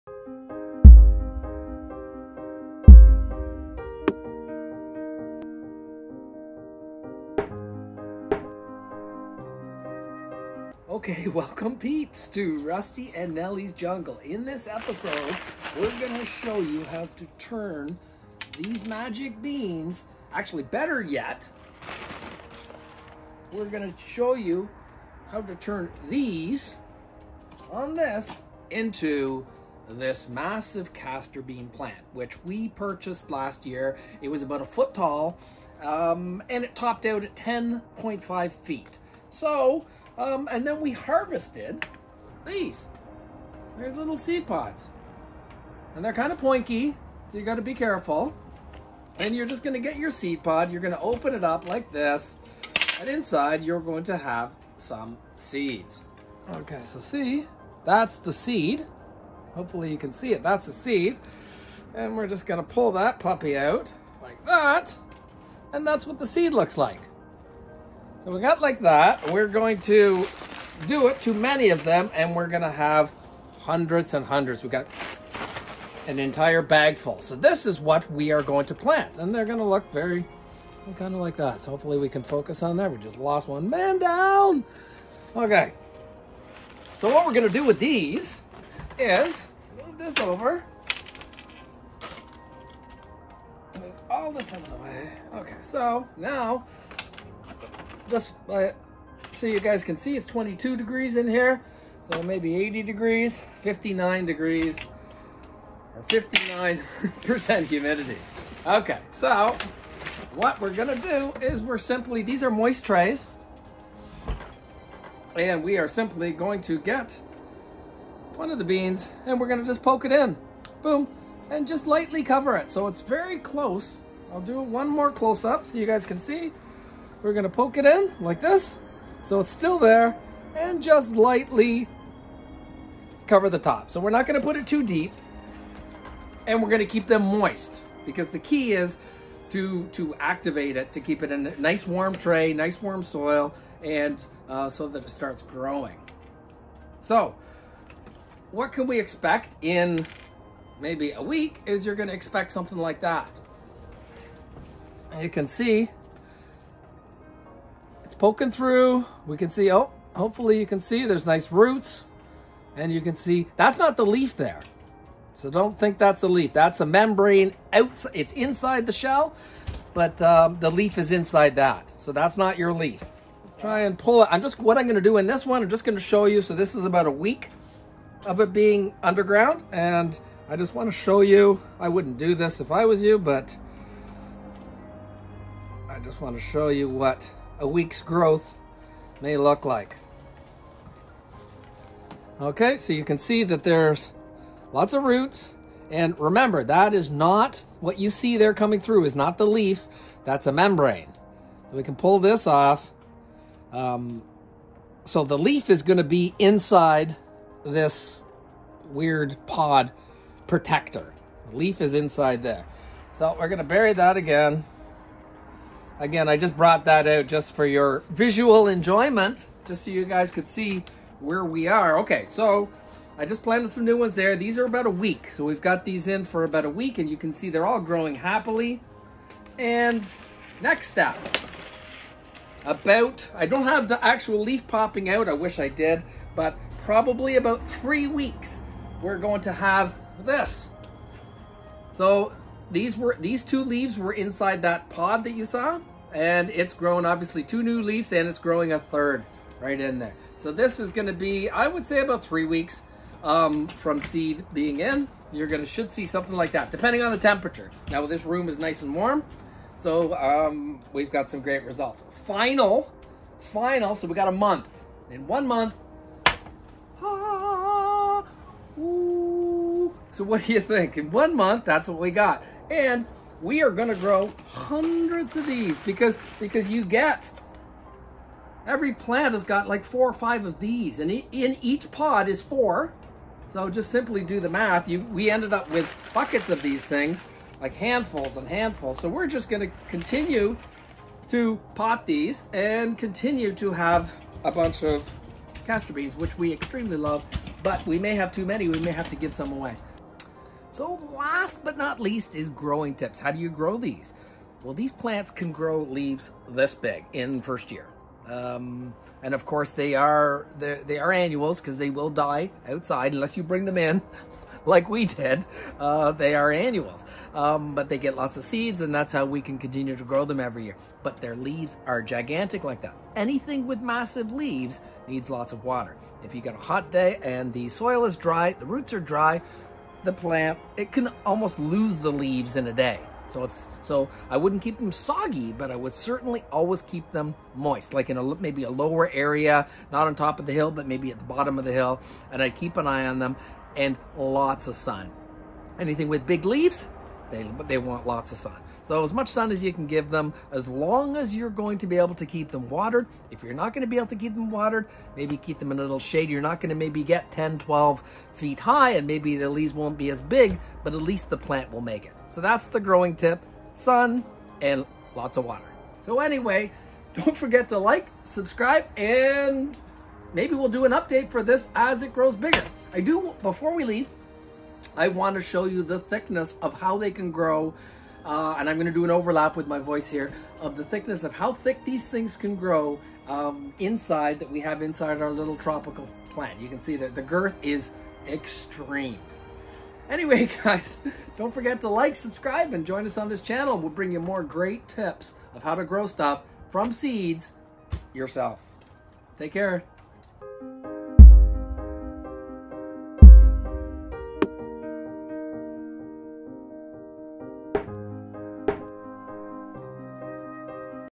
CASTER BEAN How to propagation start to finish from seed pods with RESULTS we harvest casterbean
In this video we harvest our own caster bean seeds and take you through the steps on how to soe them Step by Step . This is a DIY (Do it yourself) casterbean start to finish growing video.

(our Favorite Channels)